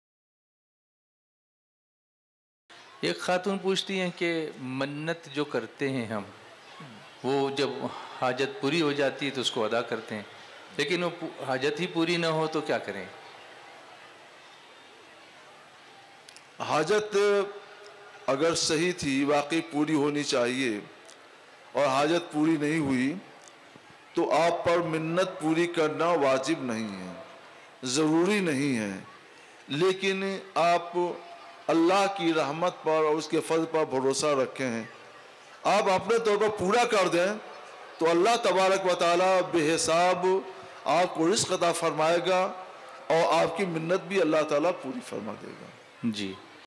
Agar mannat puri na ho to usko ada karna hai ya nahi?
Topic:Agar mannat puri na ho to usko ada karna hai ya nahi?
Orator: Mufti Nizamuddin Misbahi
Place: Jaipur